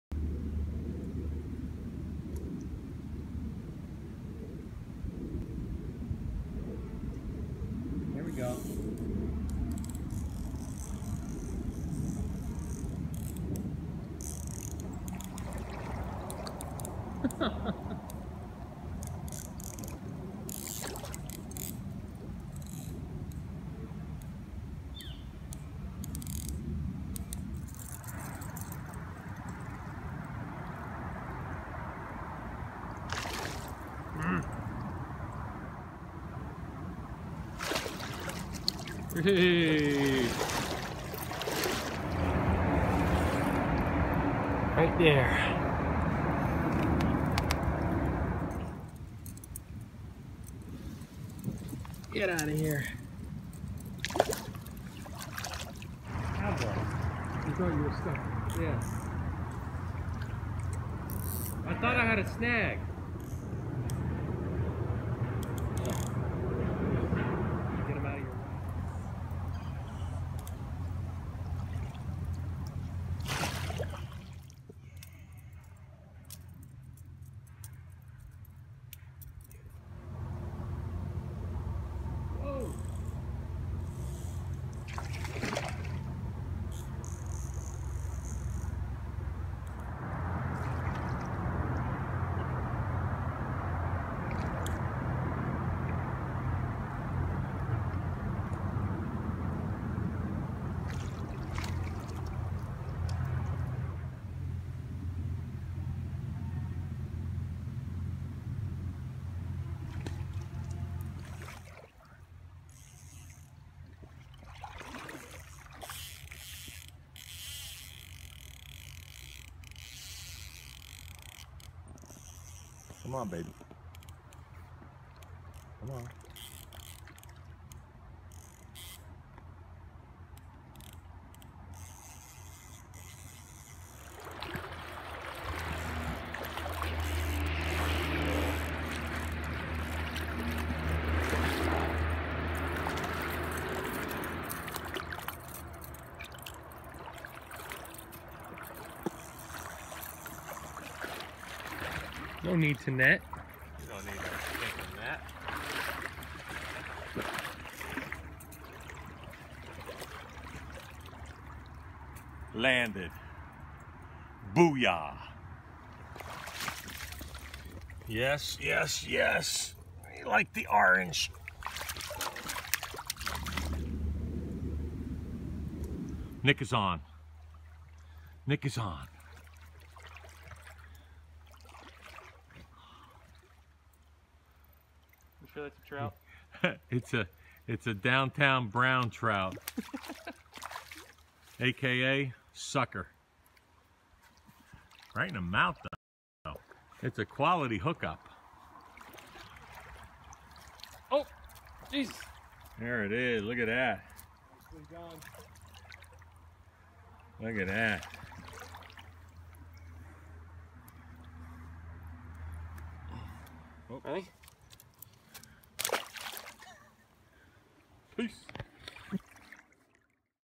Pequest River: For Suckers Only
Here's a quick video from our time fishing on the Pequest river on 11-8-17. Not a lot of trout in the river so i found a school of like 400 suckers and had some fun. My dad was the only one that day to catch a rainbow. Fun relaxing day of fishing.